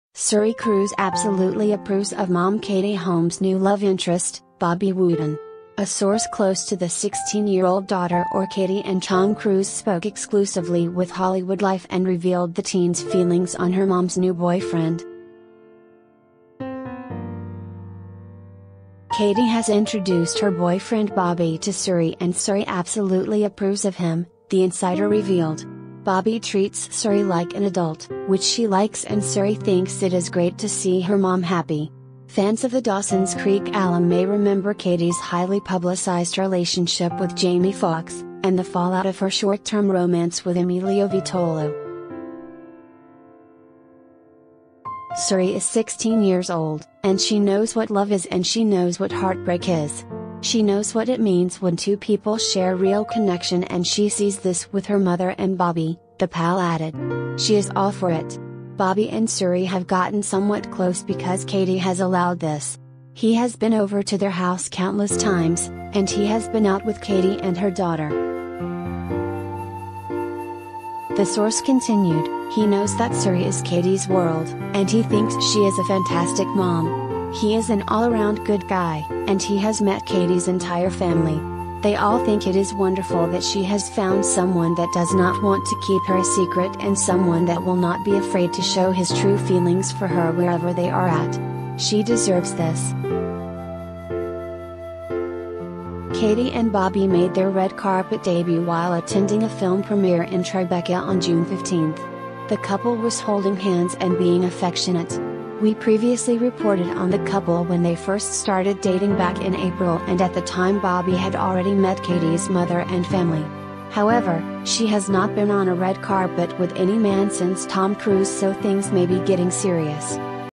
How Katie Holmes’ Daughter Suri Cruise, 16, Feels About Her Boyfriend Bobby Wooten
Suri Cruise “absolutely approves” of mom Katie Holmes‘ new love interest, Bobby Wooten. A source close to the 16-year-old daughter or Katie and Tom Cruise spoke EXCLUSIVELY with HollywoodLife and revealed the teen’s feelings on her mom’s new boyfriend.
“Katie has introduced her boyfriend Bobby to Suri and Suri absolutely approves of him,” the insider revealed. “Bobby treats Suri like an adult, which she likes and Suri thinks it is great to see her mom happy.” Fans of the Dawson’s Creek alum ma